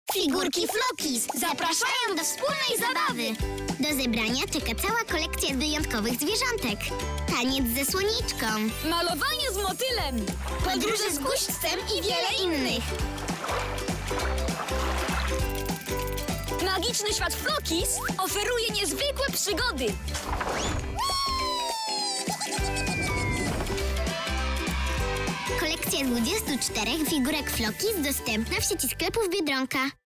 Flockies Collection Figurki Flockies SPOT REKLAMOWY
Flockies Collection figurki do kolekcjonowania dostępna w sieci sklepów biedronka